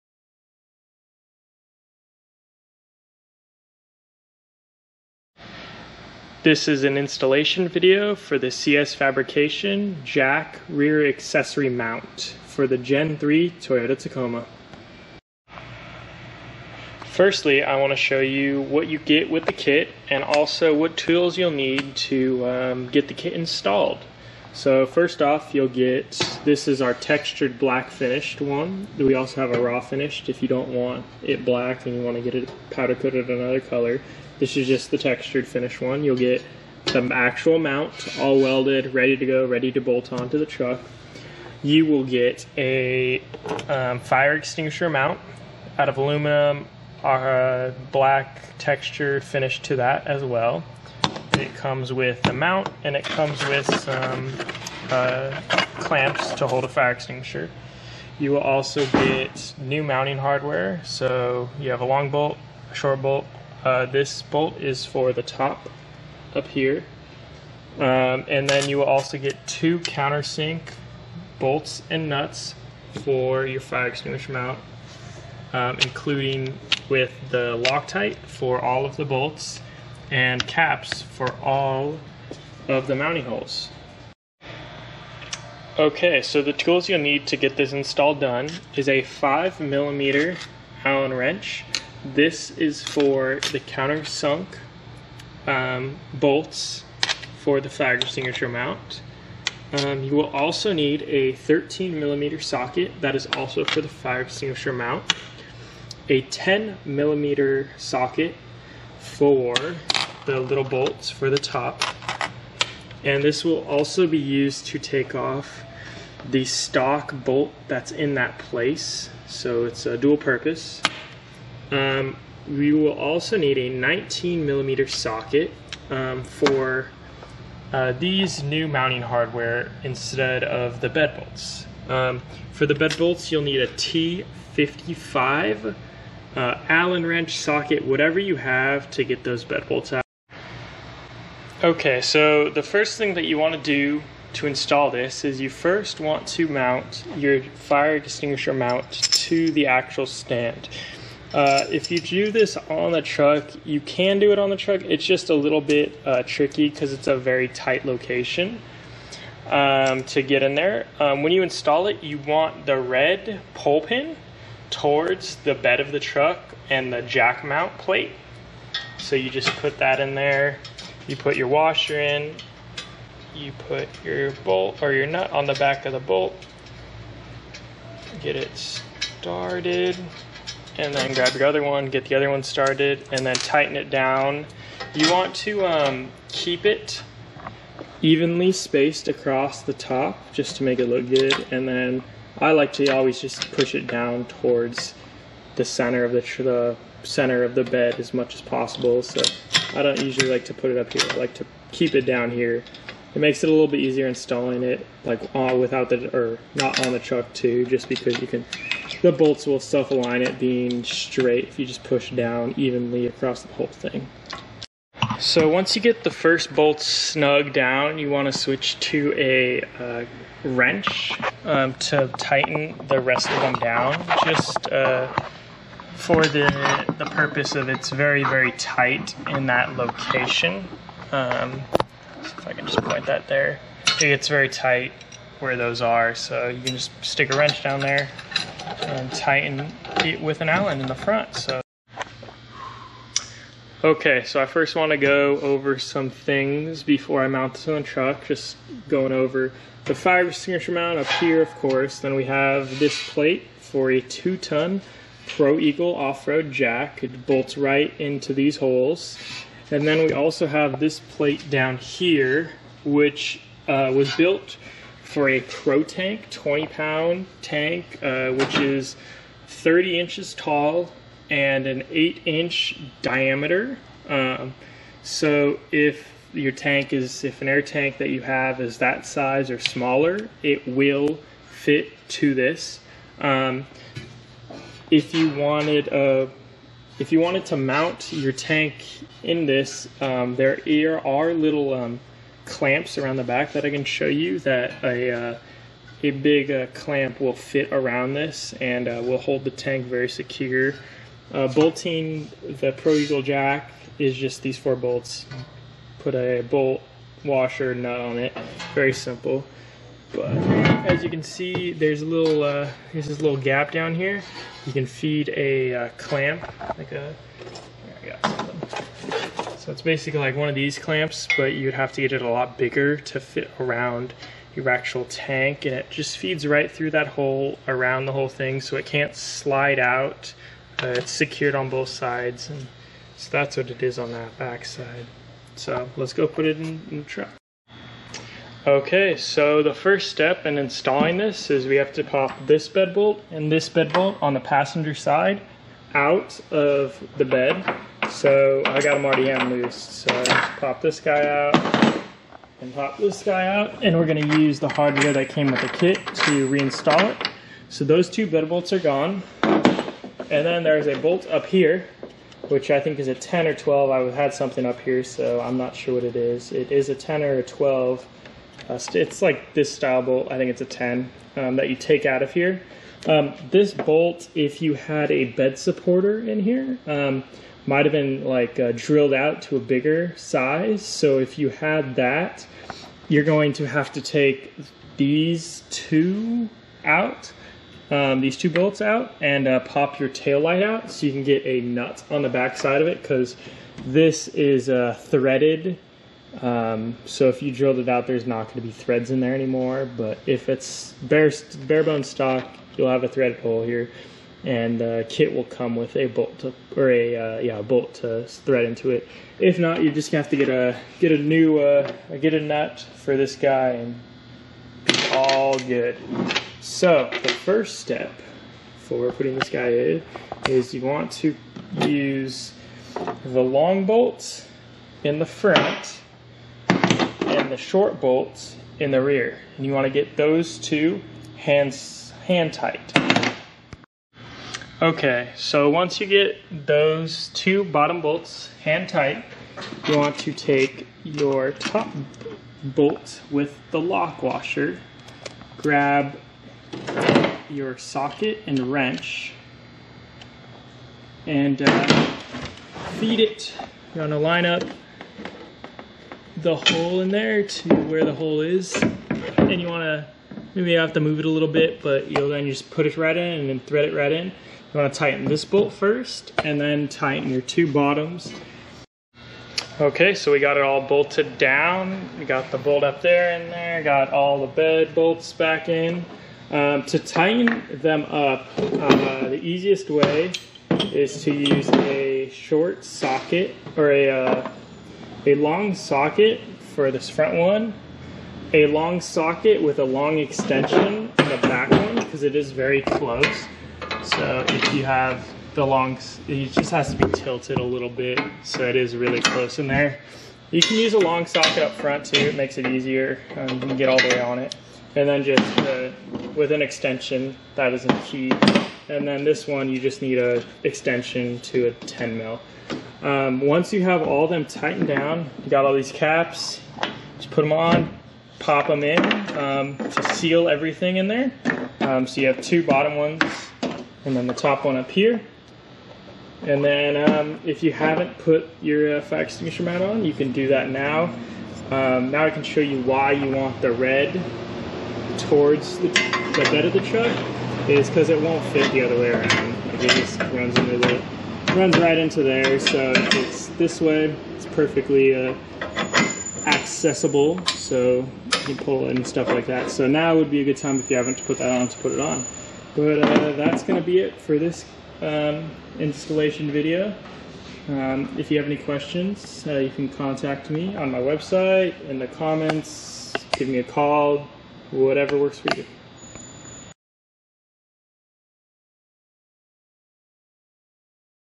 CS Fabrication Gen 3 Toyota Tacoma bolt on jack and accessories rack kit Install video.
This video show what comes in the CS Fabrication Gen 3 Toyota Tacoma bolt on jack and accessories rack kit as well as how to install the kit on your gen3 Toyota Tacoma.